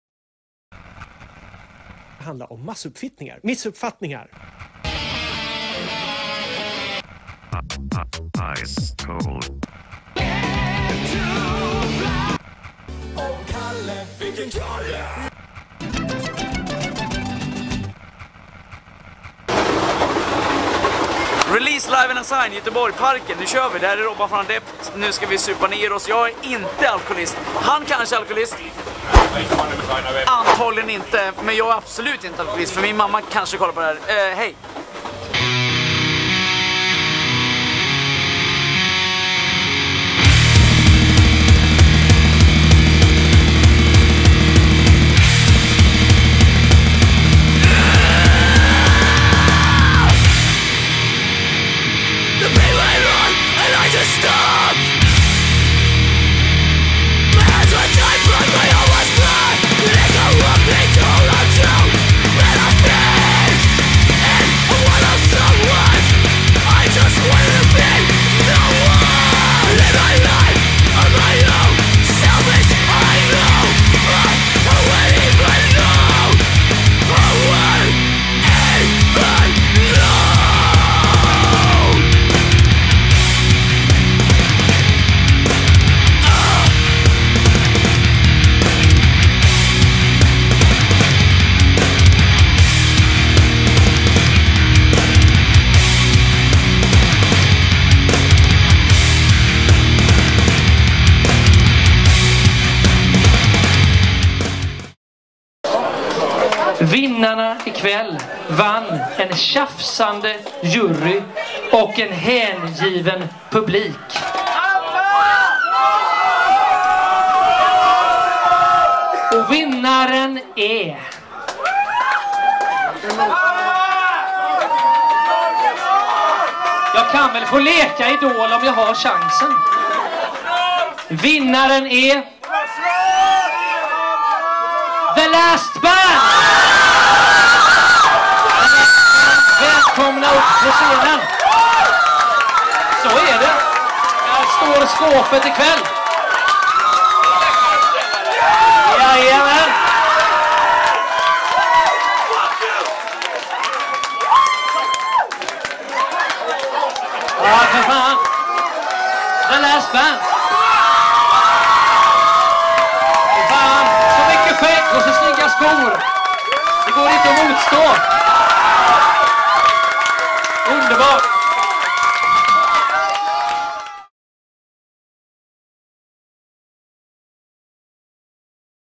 RELEASED Live and Unsigned 2012 (GBG)
RELEASED Live and Unsigned 2012
Released is back and after the success last year, we all look forward to a fantastic fall, breathing hard rock and metal!

Massive congratz to The Last Band for the third spot at the final 21/12 in Stockholm .

Pre-final Nr.3 at Debaser , GBG .
Bands:
Meadows End
Scandinavian Hateland
Demotional
The Last Band